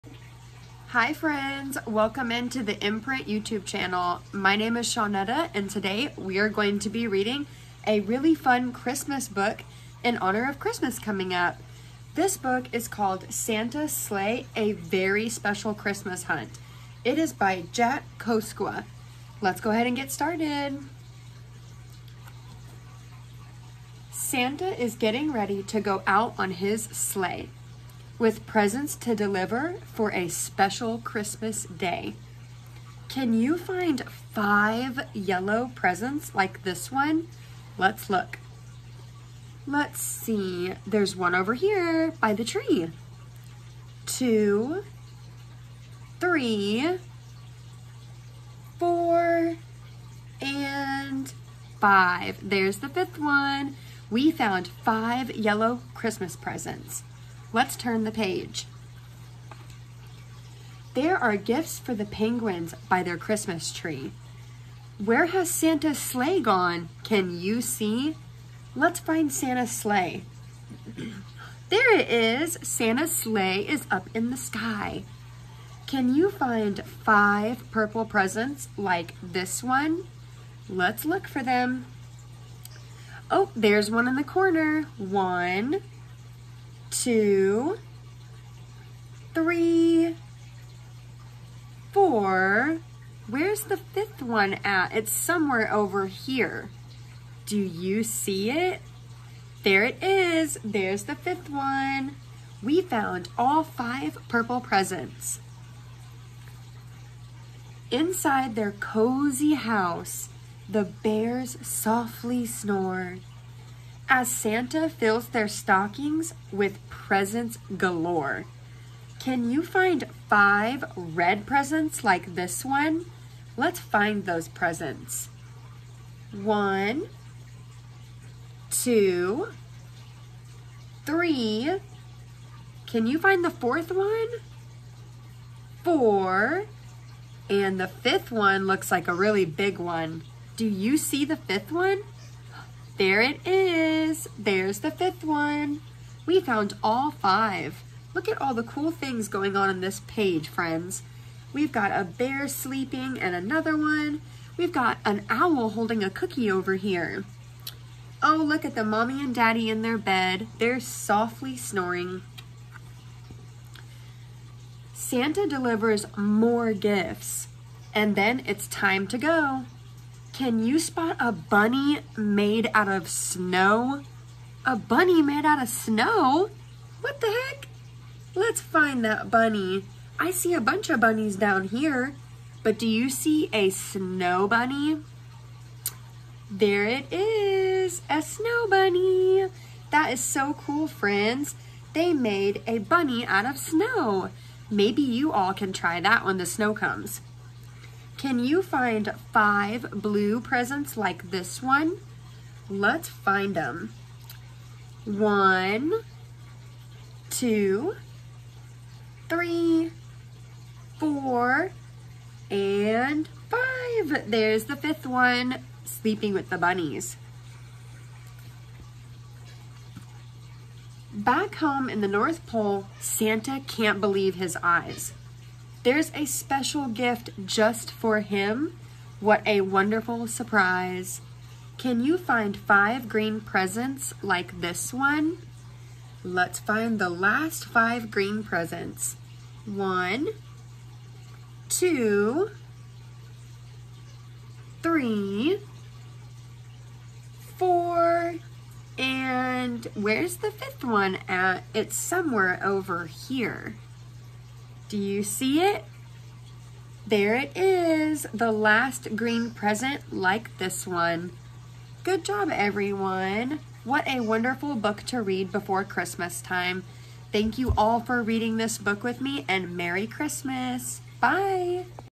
Santa’s Sleigh a Very Special Christmas Hunt
Santa’s Sleigh a Very Special Christmas Hunt reading by Shawnetta